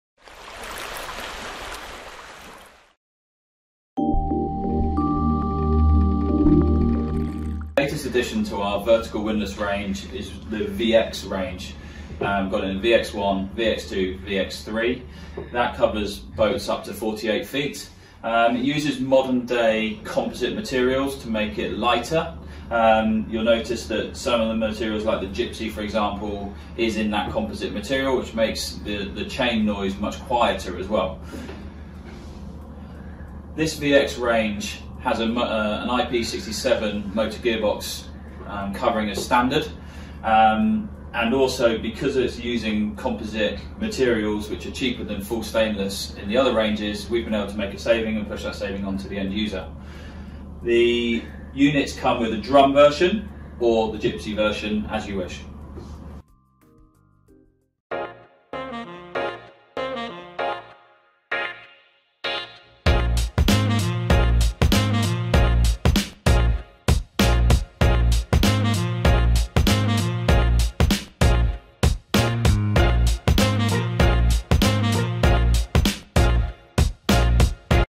Lewmar VX windlass
Lewmar VX windlasses have composite gypsies for less noise